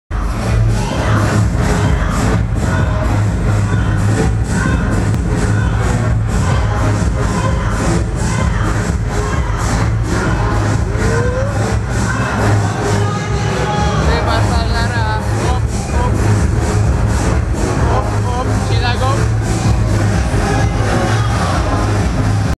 Ренат @ Arena 2016
Известный клубный звуко и светотехник Казани Ренат за работой в Арене (2016)